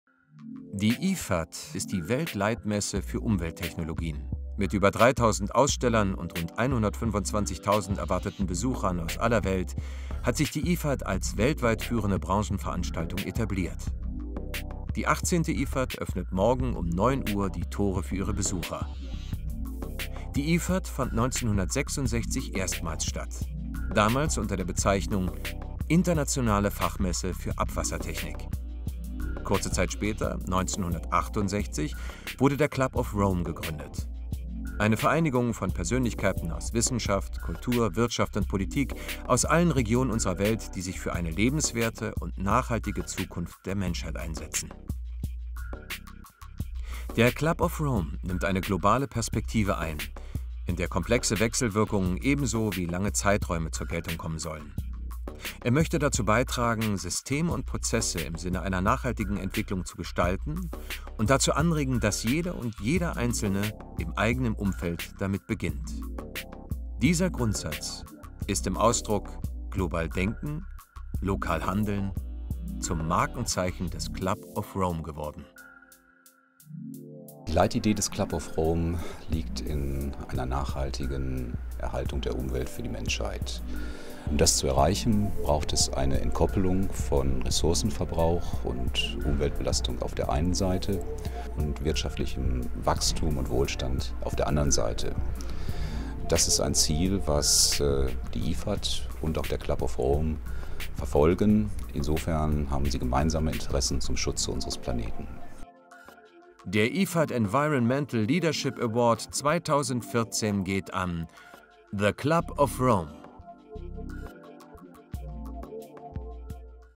GreenTec Awards 2014 Trailer IFAT Environmental Leadership Award
Der Trailer stellt den Preisträger des IFAT Environmental Leadership Awards im Rahmen der GreenTec Awards 2014 filmisch vor.
Preisträger: The Club of Rome.
Die Trailer wurden von der Berliner Produktionsfirma Pape Schürmann produziert.